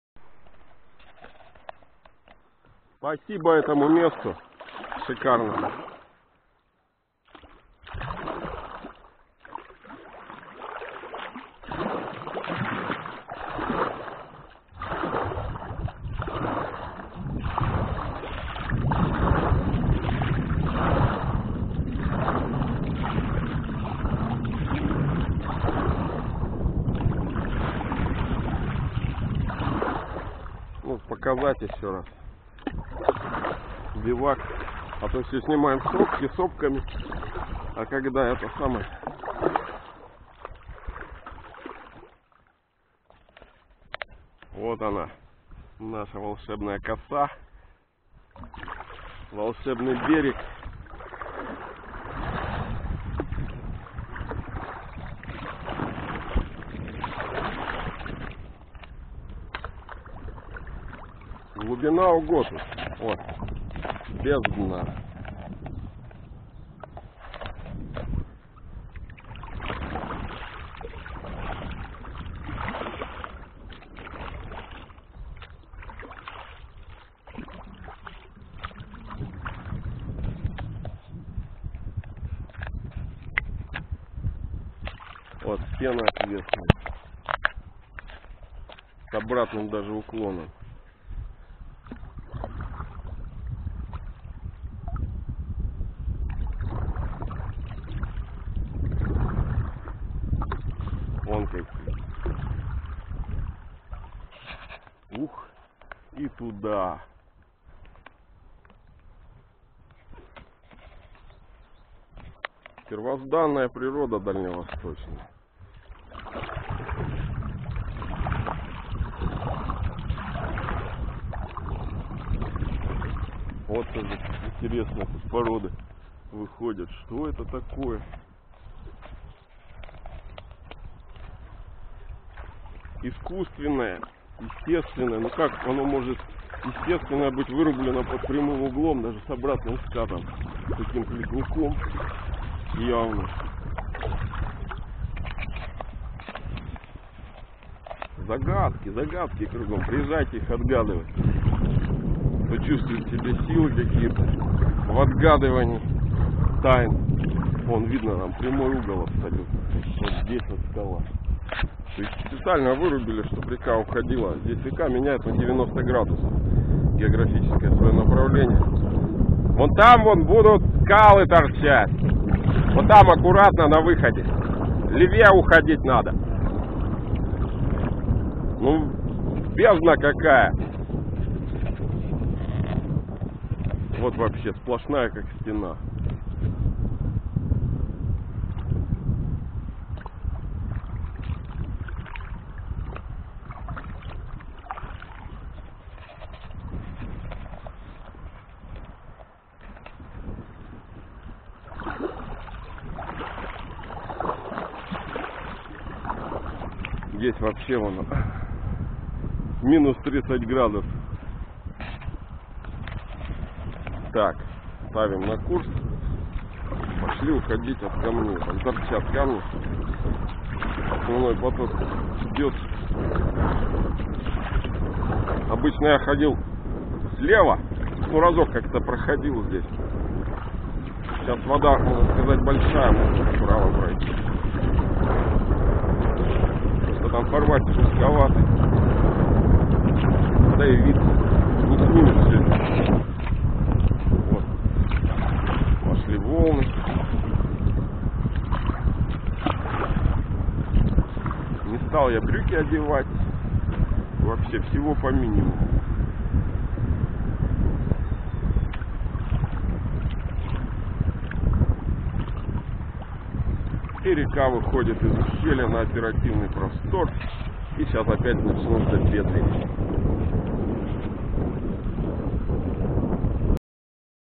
Стена на реке Хунгари
Загадочный хребет, завершающий "страну пирамид", ровный как стена, и вытянутый в широтном направлении, изменяющий направление течение реки Хунгари на 90 градусов - с южного на западное.